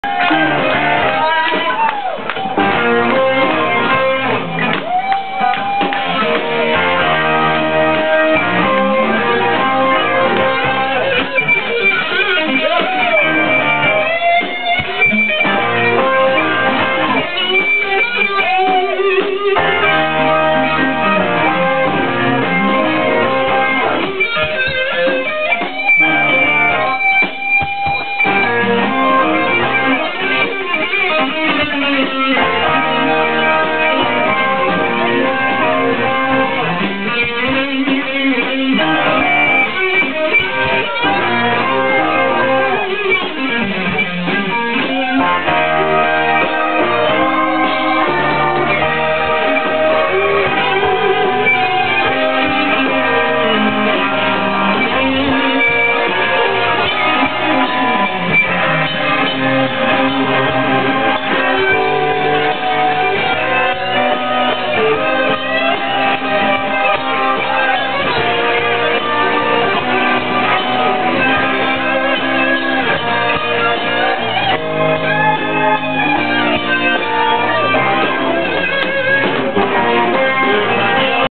Asoka - Video 1
Nice groove from the Swedish band ASOKA. (From the Mellotronen´s 20:th anniversary, 27/1 -2007).